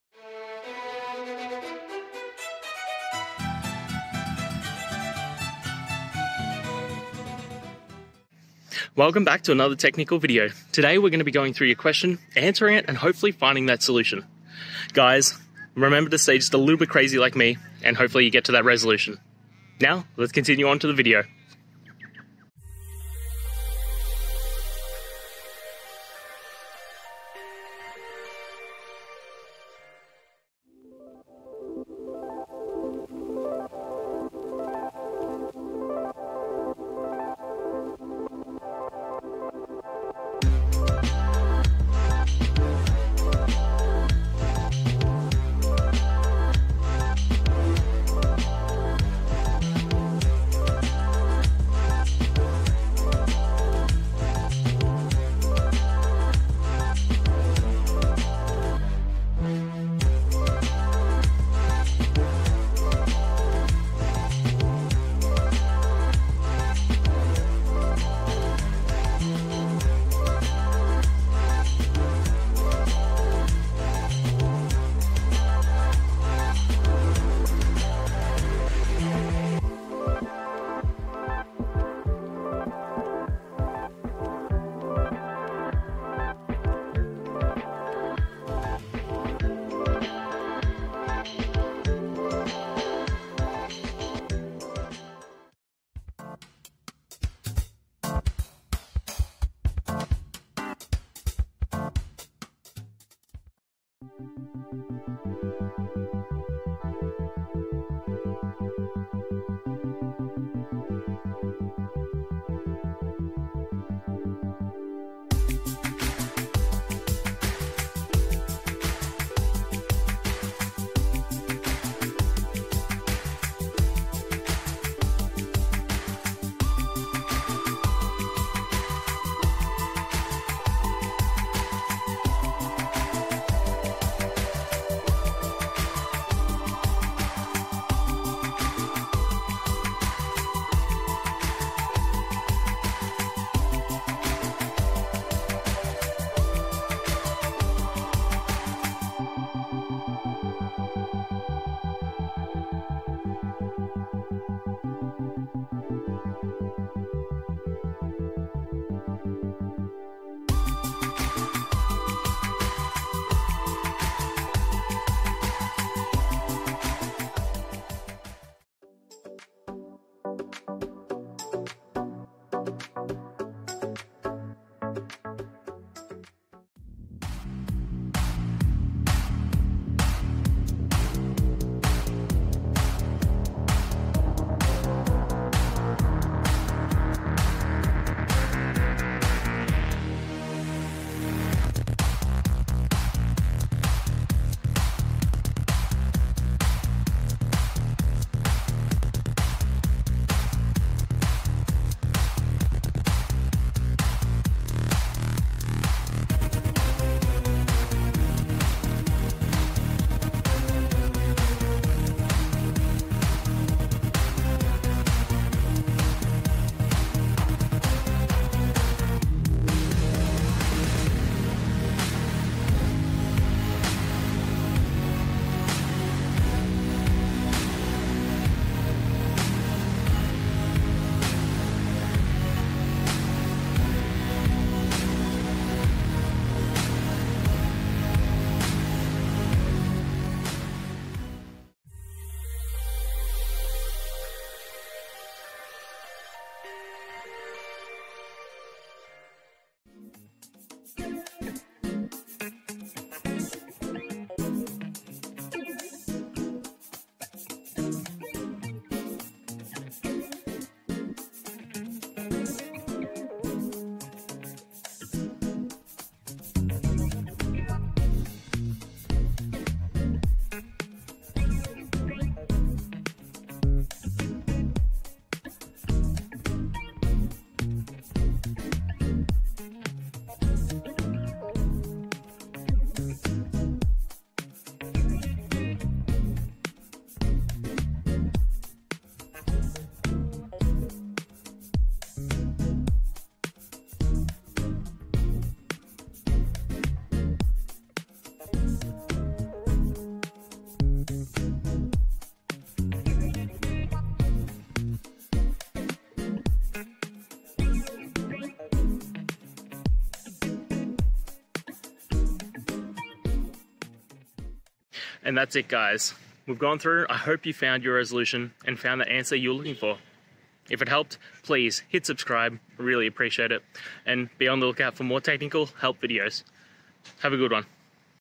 Styling React Navigation Tabs for Android: A Complete Guide
In this video, we dive into the world of React Navigation and explore how to style tabs specifically for Android applications. Whether you're a beginner or an experienced developer, you'll learn essential techniques to customize the look and feel of your navigation tabs, ensuring a seamless user experience. Join us as we provide a step-by-step guide filled with tips and best practices to elevate your app's design!

Today's Topic: Styling React Navigation Tabs for Android: A Complete Guide

Related to